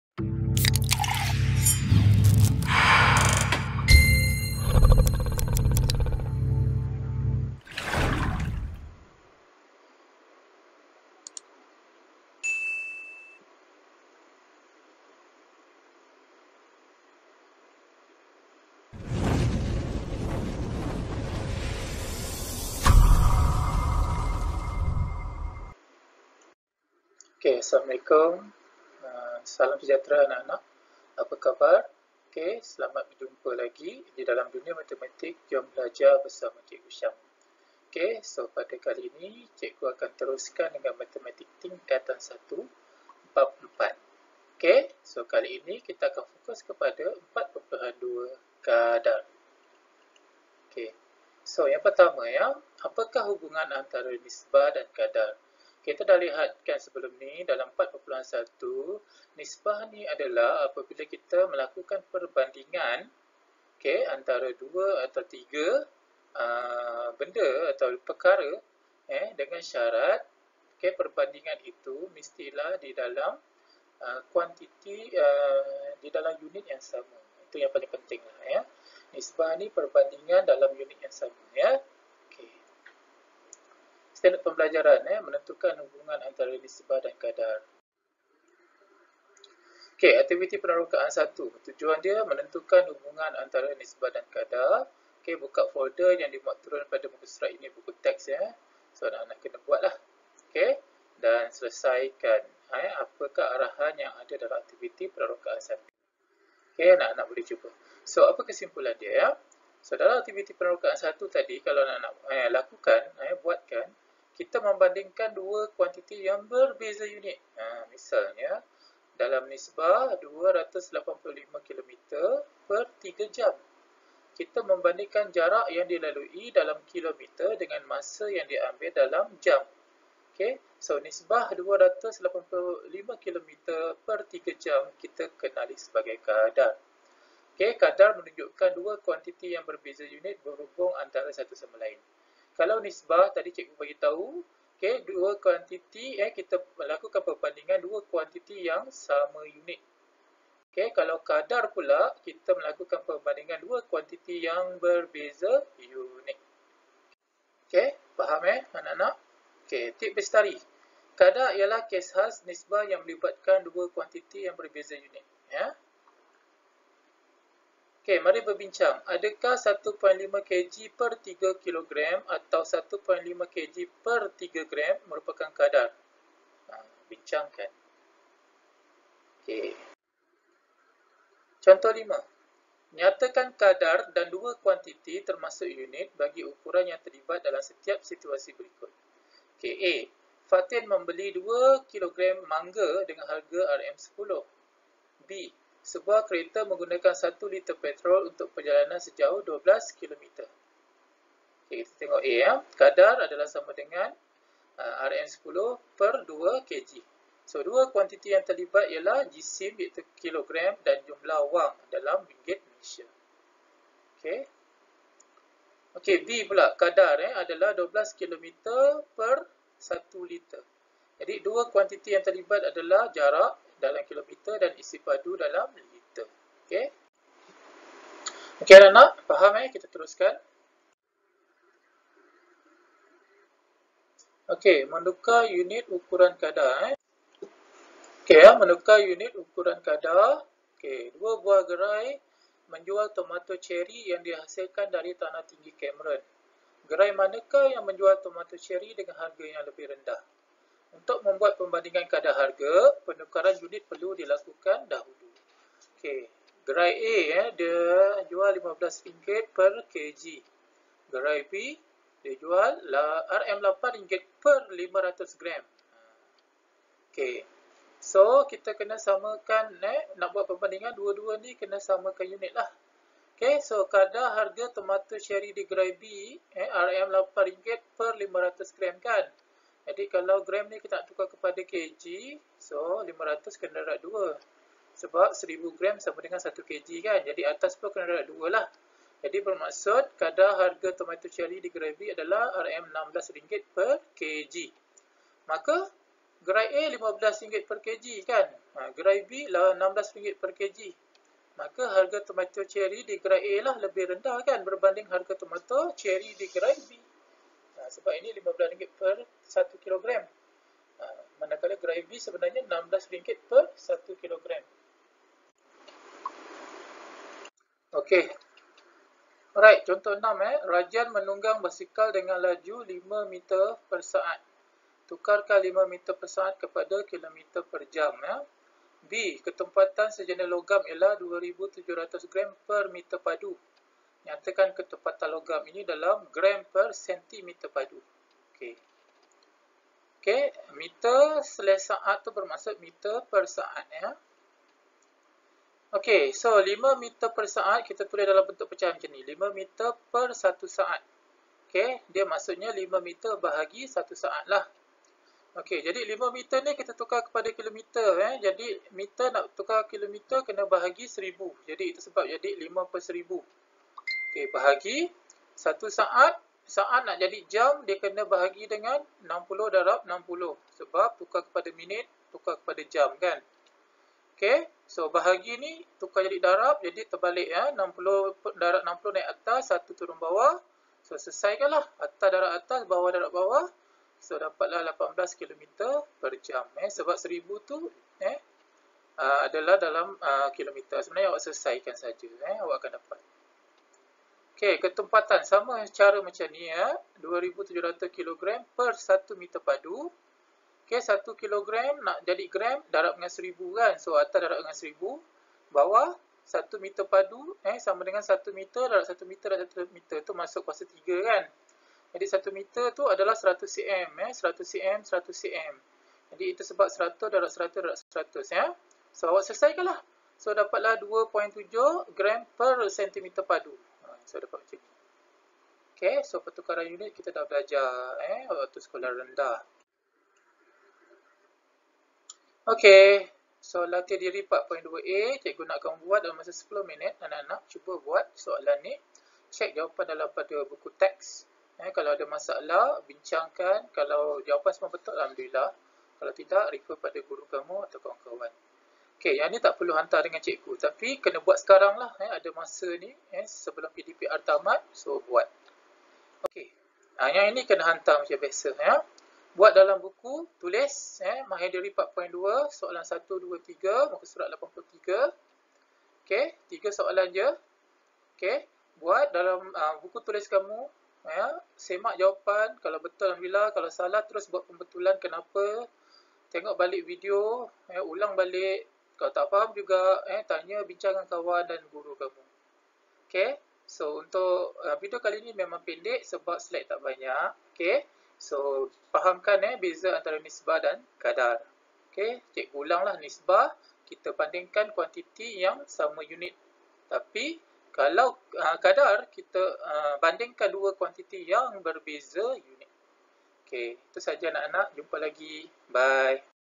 MM T1 BAB 4 4 2 Kadar
DUNIA MATEMATIK... JOM BELAJAR MATEMATIK BERSAMA CIKGU SHAM..
MATEMATIK TINGKATAN 1
Bab 4 Nisbah, Kadar dan Kadaran
4.2 KADAR